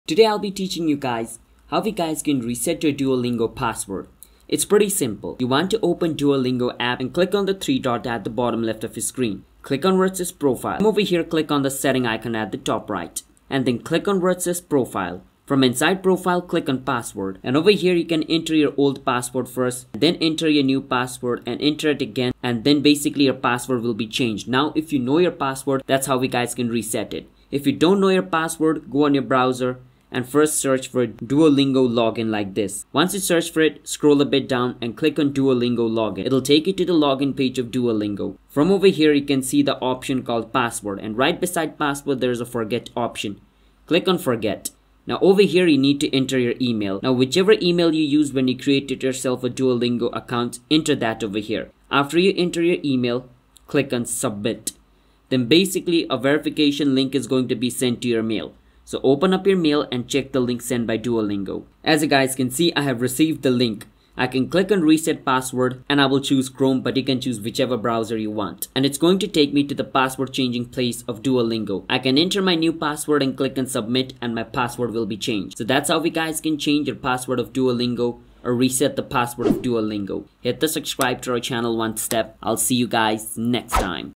How To Reset Duolingo Password (EASY!) | Recover Duolingo Password Help
This video walks you through the step-by-step process of how to reset Duolingo password. Watch the video till the end and learn the steps in detail.